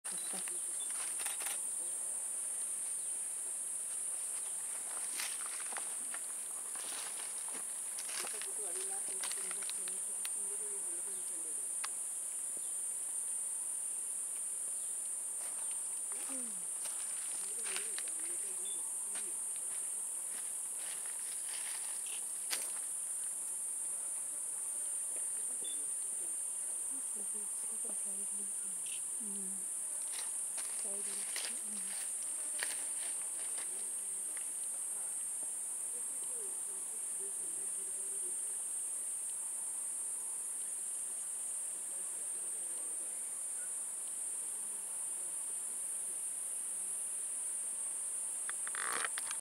Short-horned Grasshopper insect
Shrikant Kelkar's visit to Phansad WLS in Maharashtra, India

 Family - Acrididae